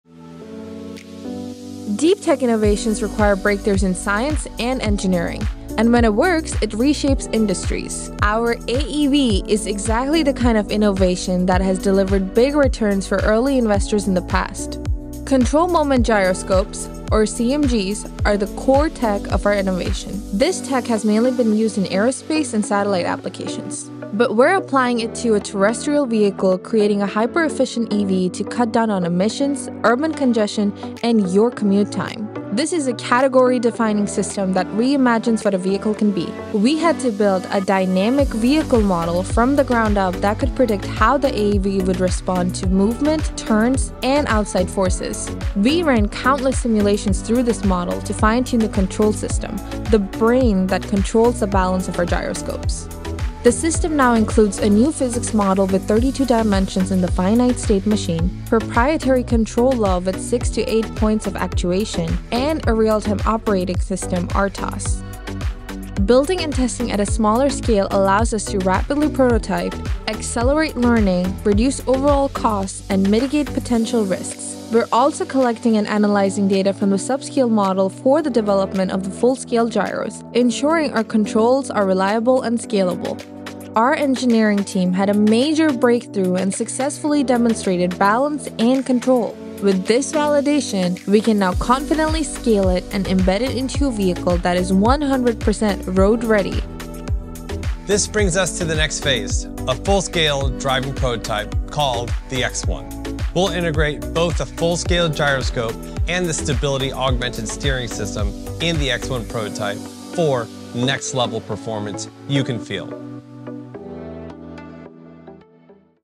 Deep Tech
Lit Motors is creating entirely new technology from the ground up—placing us in a category known as deep tech. Where traditional automakers iterate, we innovate.

Deep tech refers to innovations built on advanced science and engineering—solutions that take years of R&D, tackle complex technical challenges, and have the potential to disrupt entire industries.

Our AEV is a prime example. From its self-balancing gyroscopic system to its compact, high-efficiency drivetrain, we’re not building on existing vehicle platforms—we’re inventing our own.

In this video, we dive into what makes our core technology so unique—and why we committed to the deep tech path from day one.